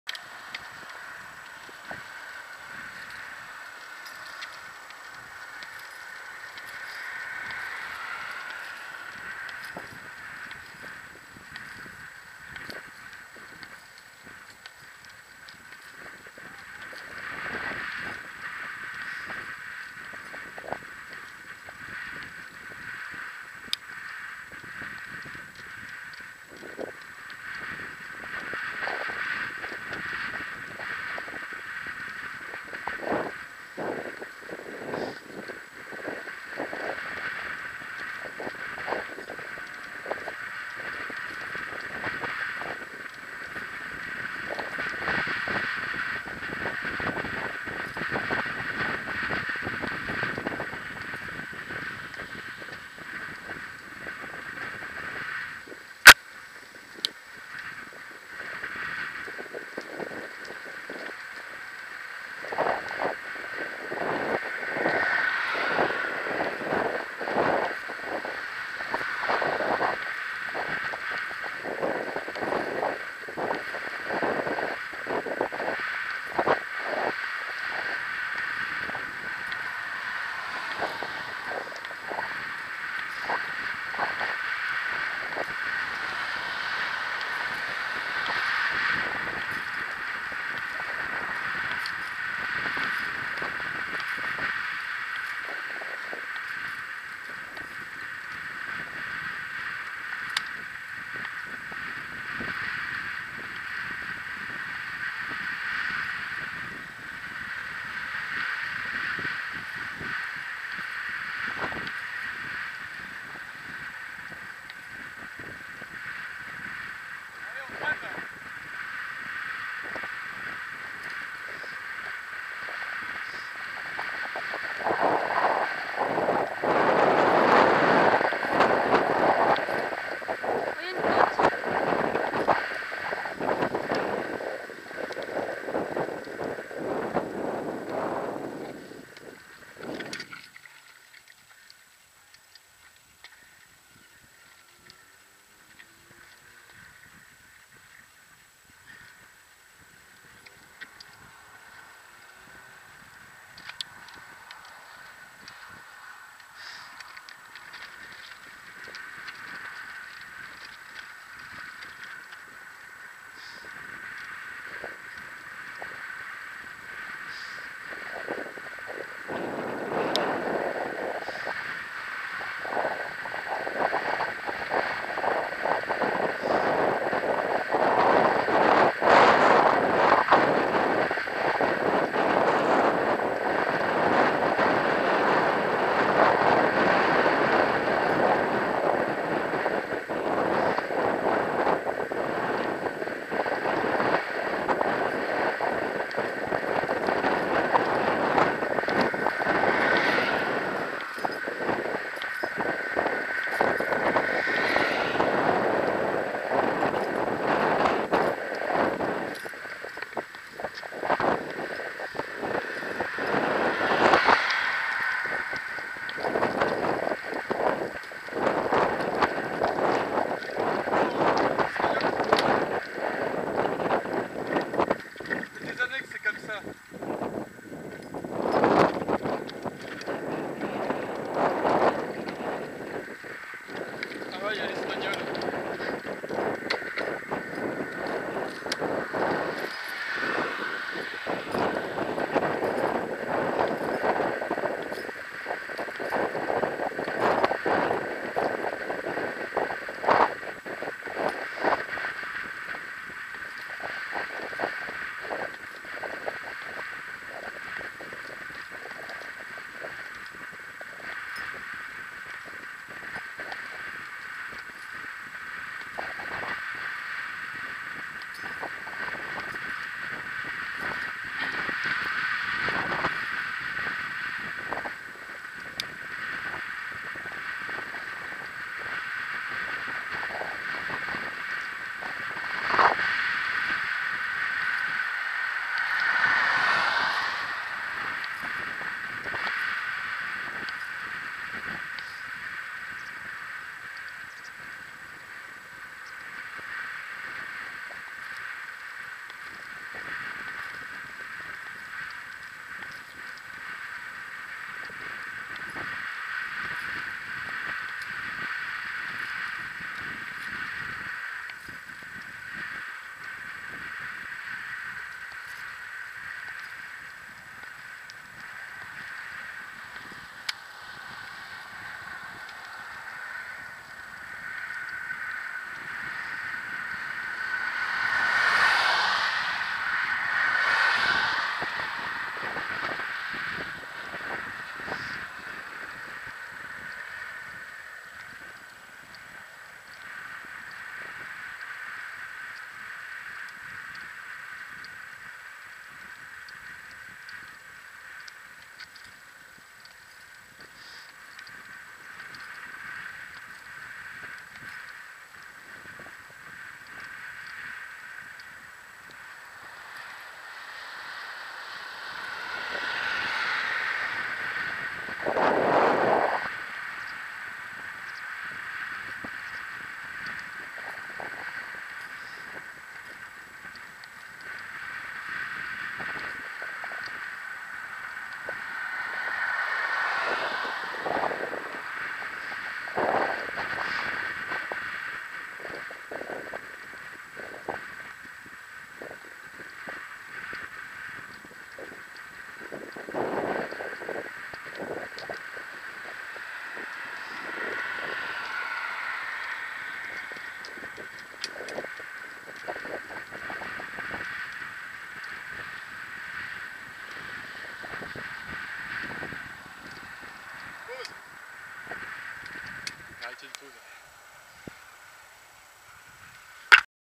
Alicante - Elche - Alicante on a skateboard Part 7
So many roads to cross and almost no indicators on the bike path.